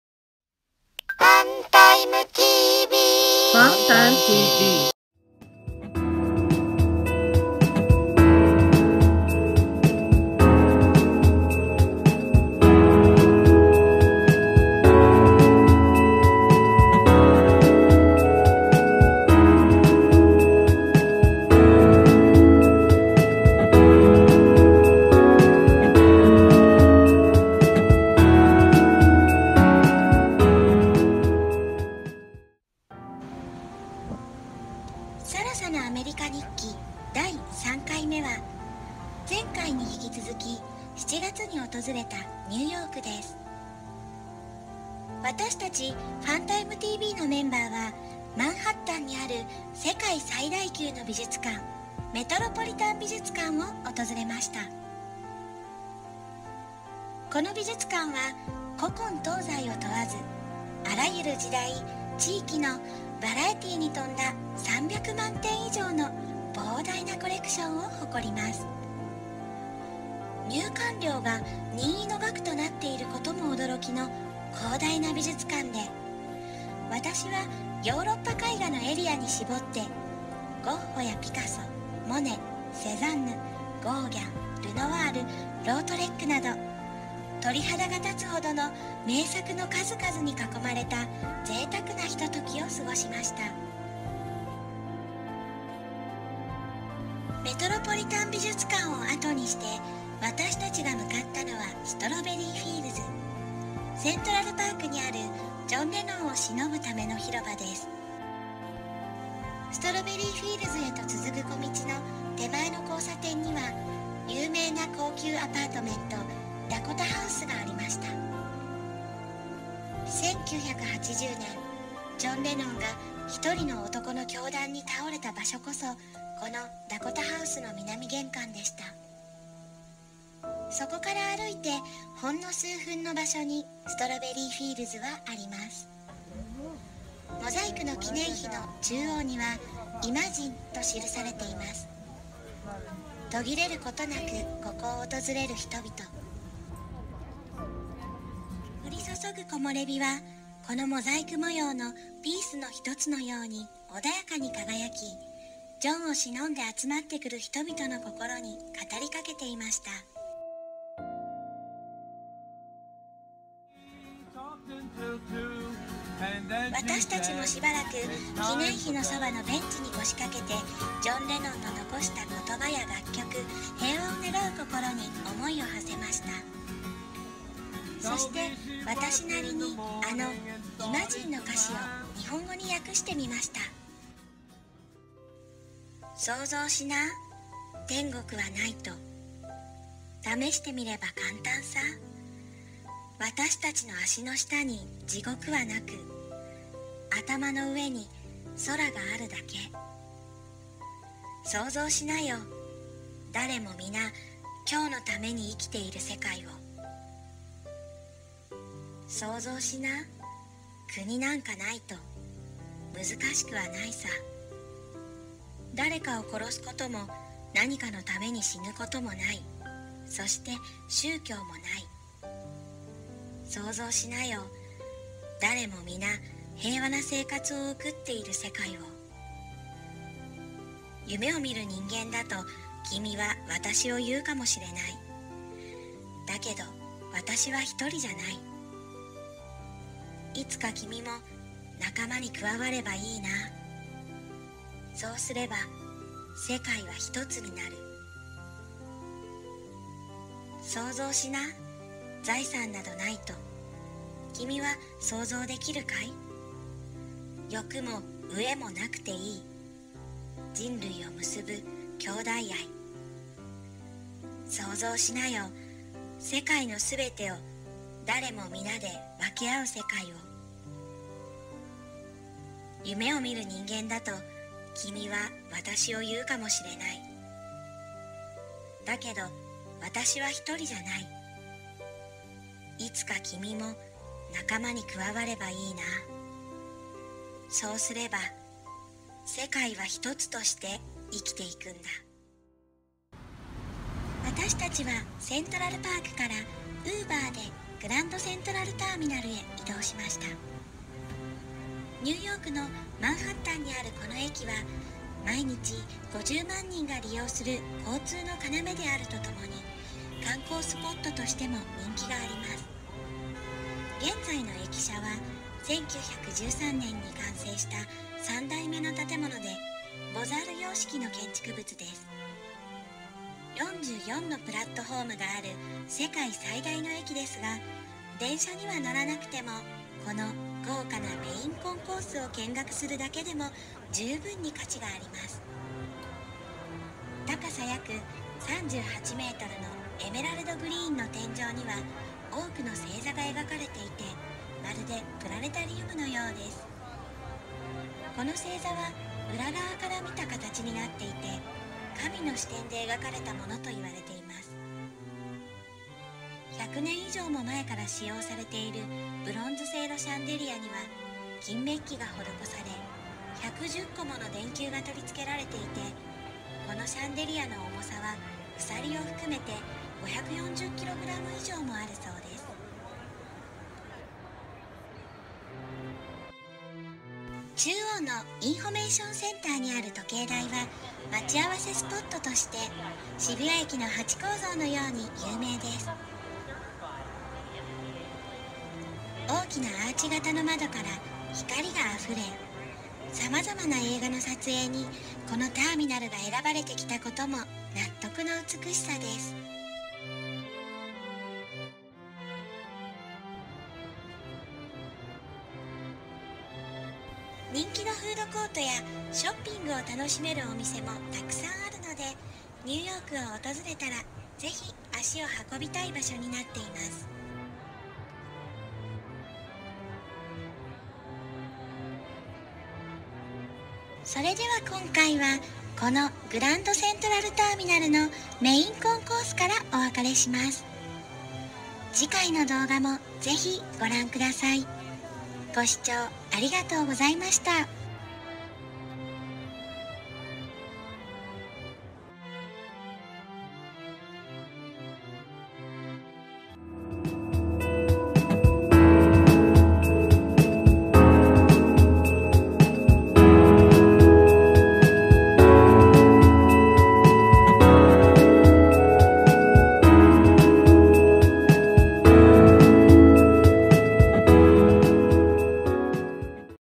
【ニューヨーク】セントラルパークとグランドセントラル駅
さらさのアメリカ日記第３回目は、ニューヨークのセントラルパークにあるストロベリーフィールズを訪れます。グランドセントラルターミナルのメインコンコースの様子もちょっぴりお届け。